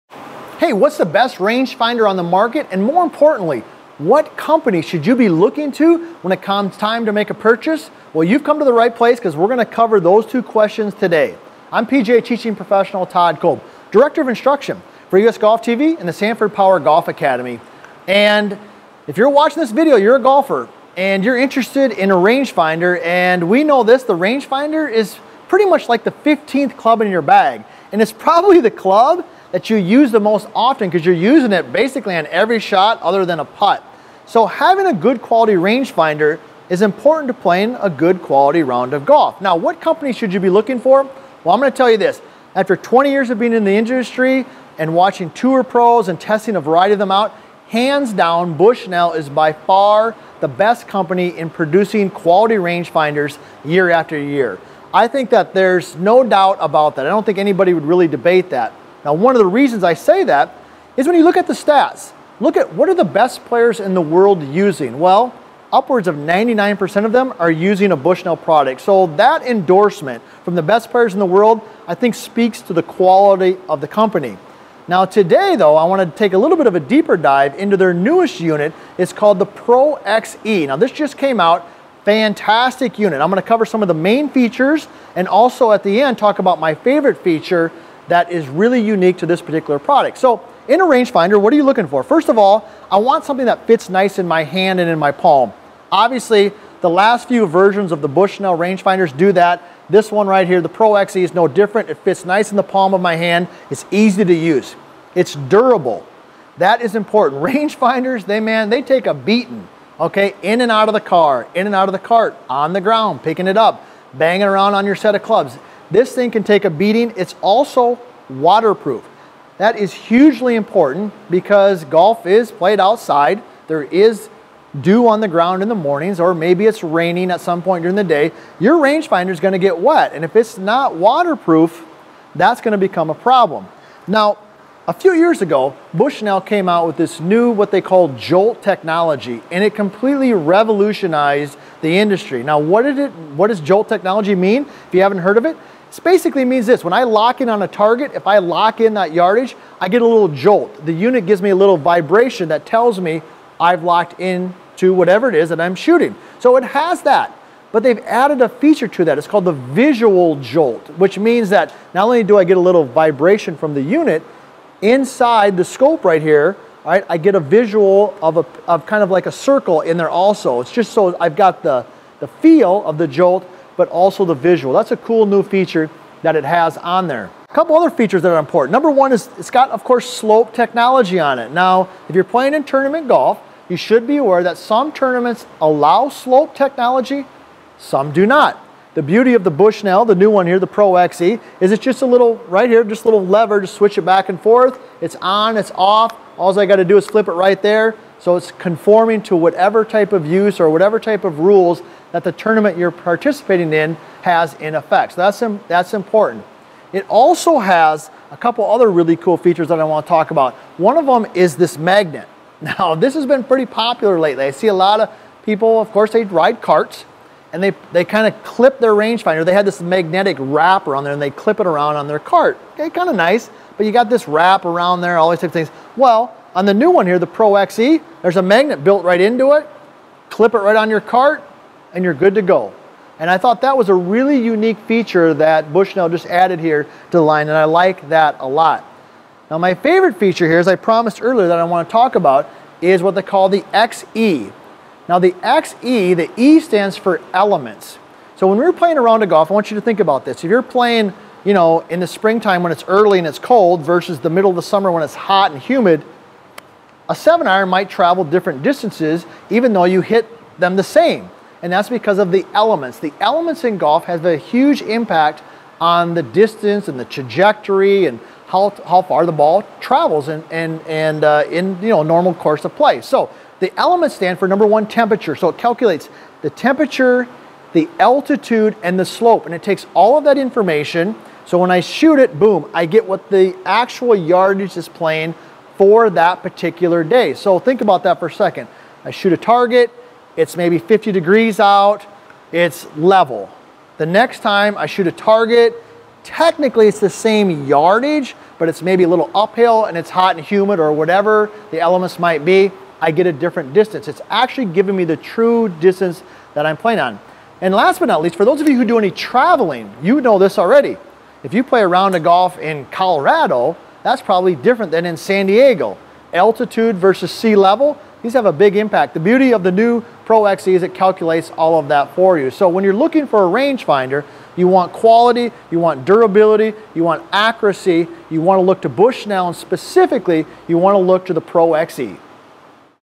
Bushnell Pro XE Review
Having a quality rangefinder is important to having a good quality round of golf.  Bushnell is by far the best company at producing quality rangefinders year after year.  I think that there’s no doubt about that.  Now today though, I want to take a little deeper dive into their newest unit, it’s called the Bushnell Pro XE.

Now a few years ago Bushnell came out with what they call JOLT Technology, and it completely revolutionized the industry.  Now what is JOLT Technology mean?  If you’ve never heard of it, it basically means this:  if I lock in on a target, if I lock in that yardage, I get a little jolt.  The unit gives me a little vibration that tells me I’ve locked into whatever it is that I’m shooting.  So it has that, but they’ve added a feature to it, called a Visual JOLT, which means not only do I get a little vibration from the unit, inside the scope right here I get a visual of a circle in there also.  So I’ve got the feel of the JOLT, but also a visual.  That’s a cool new feature that it has on there.

A couple other features that are important.  It’s got of course Slope Technology on it.  If you’re playing in tournament golf, you should be aware that some tournaments allow slope technology, some do not.  The beauty of the Bushnell--the new one here, the Pro XE--it’s just a little lever right here to switch it back and forth.  It’s on, it’s off, all I’ve got to do is flip it right there so it’s conforming to whatever type of use or whatever type of rules that the tournament that you’re participating in has in effect.  So that’s important.

It also has a couple other really cool features that I want to talk about.  One of them is this magnet.  Now this has been pretty popular lately.  I see a lot of people that, of course, they ride carts, and they kind of clip their rangefinder--they have this magnetic wrap around there--and they clip it on their cart.  Kind of nice, but you’ve got this wrap around there.  Well on the new one here, the Pro XE, there’s a magnet built right into it, clip it right onto your cart and you’re good to go.  And I thought that was a really unique feature that Bushnell just added here to the line, and I like that a lot.

Now my favorite feature here--as I promised earlier--that I want to talk about is what they call the XE.  Now the XE, the E stands for Elements.  So when you’re playing a round of golf, I want you to think about this.  If you’re playing in the springtime when it’s early and it’s cold, versus the middle of the summer and it’s hot and humid, a 7 iron might travel different distances even though you hit them the same.  And that’s because of the elements.  The elements in golf have a huge impact on the distance and the trajectory and how far the ball travels in normal course of play.

So the Elements stand for number 1, temperature.  So it calculates the temperature, the altitude and the slope, and it takes all of that information, so when I shoot it I get what the actual yardage is playing for that particular day.  So think about that for a second.  I shoot a target, it’s maybe 50 degrees out, it’s level.  The next time, I shoot a target, technically it’s the same yardage, but it’s maybe a little uphill and it’s hot and humid, or whatever the elements might be.  I get a different distance.  It’s actually giving me the true distance that I’m playing on.

- for more great articles, tips, product and reviews

FIX YOUR SLICE TODAY!